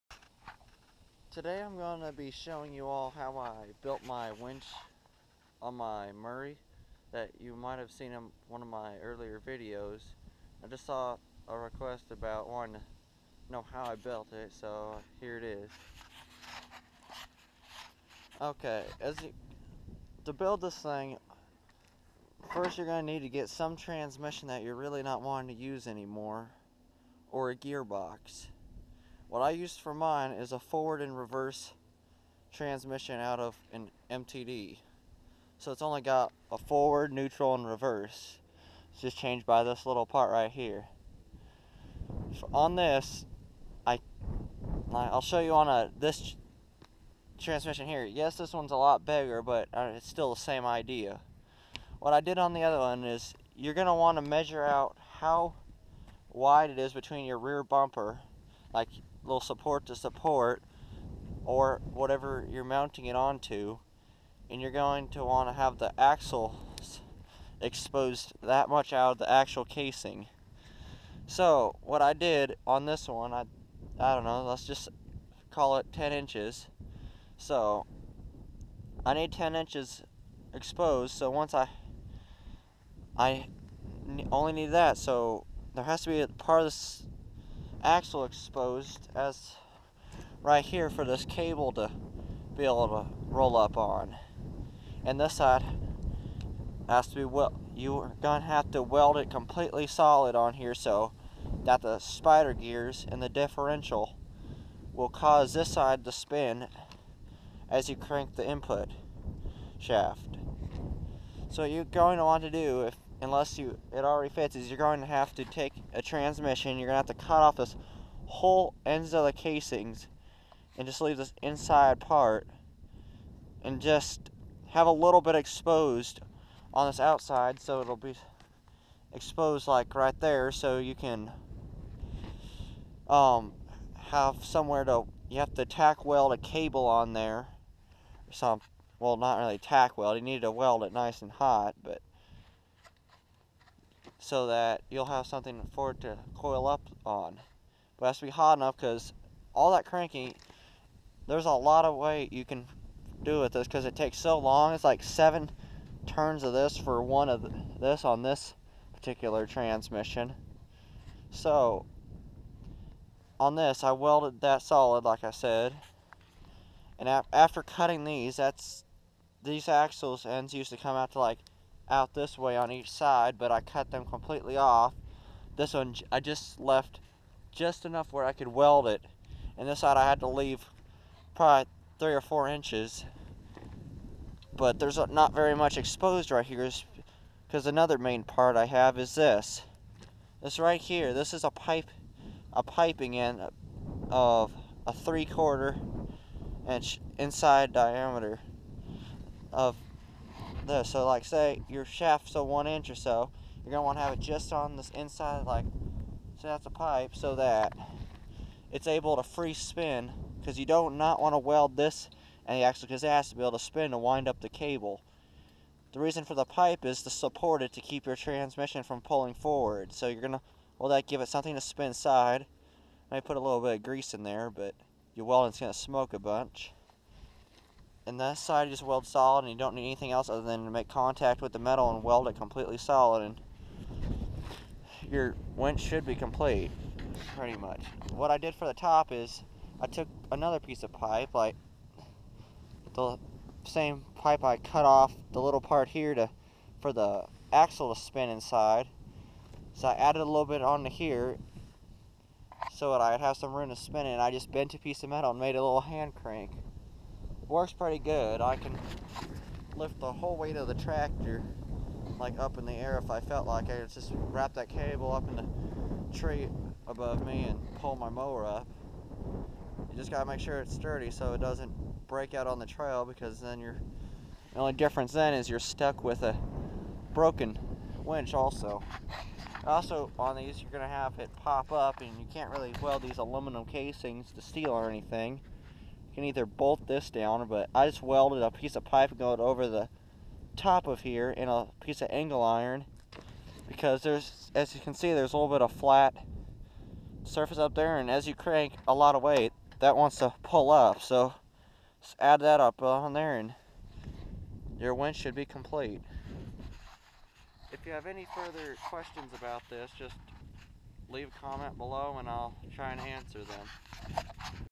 How to build a winch on your offroad mower
just a use for a junk transmission